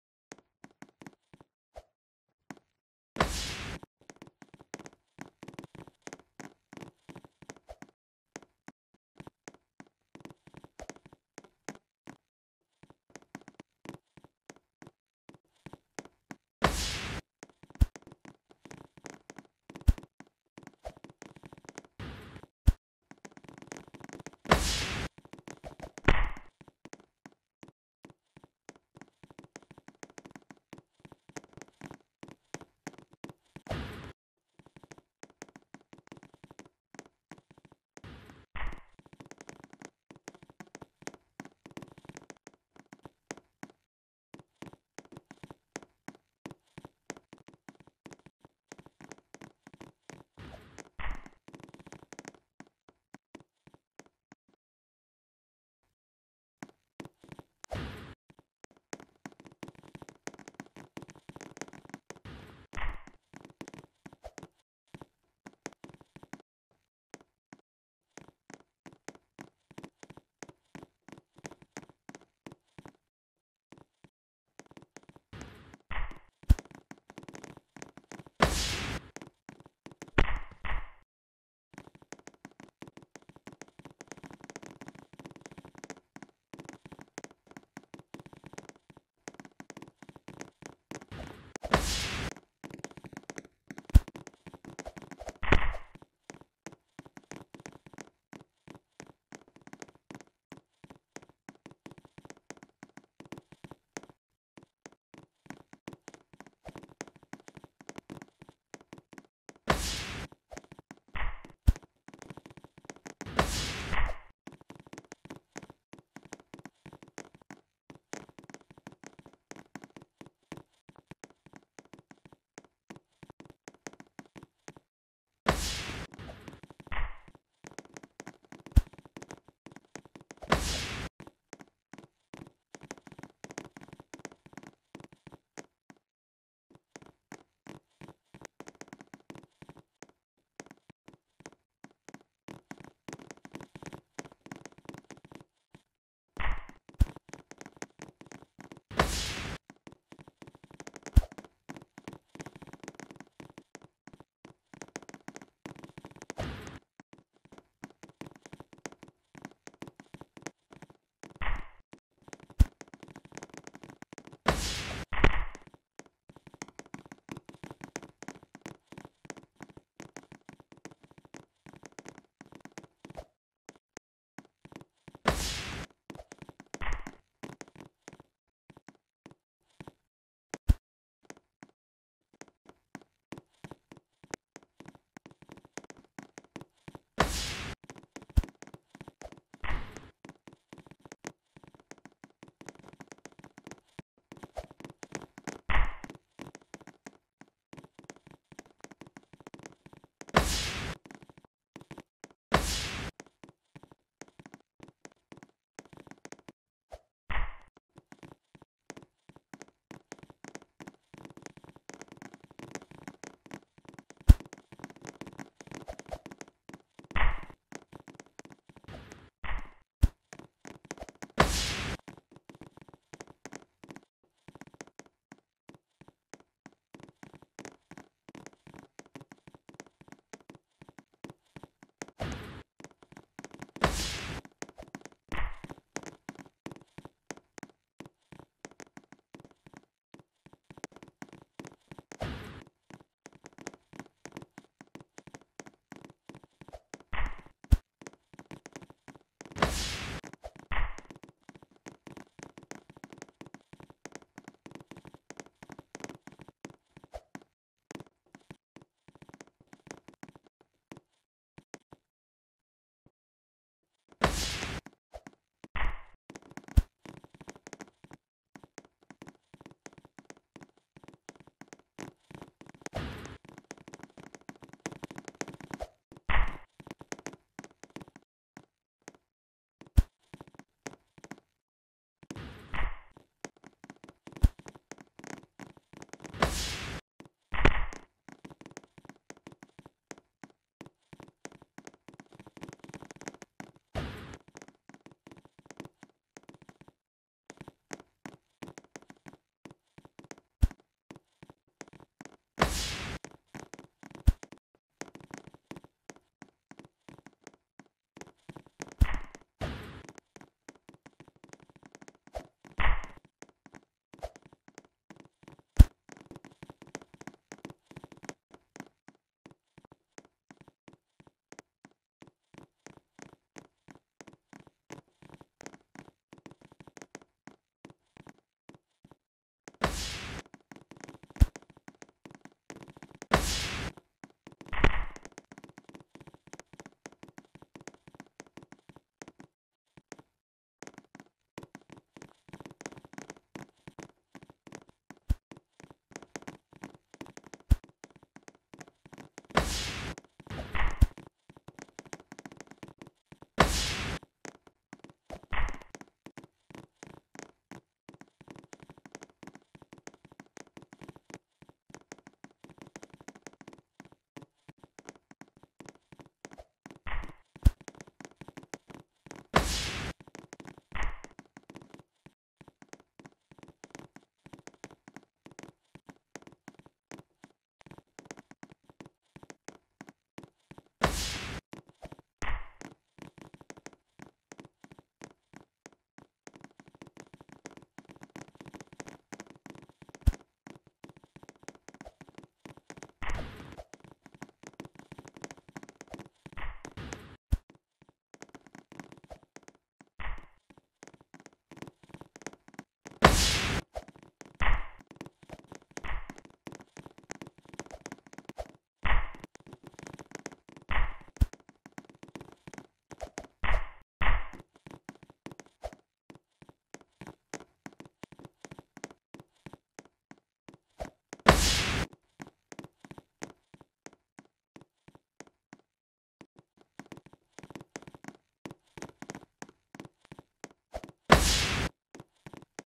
my team pulled a really good comback in volleyball 4.2 (sadly not me)
also congrats for my team for pulling that comeback and i wasnt playing because the other team wanted us to have equal players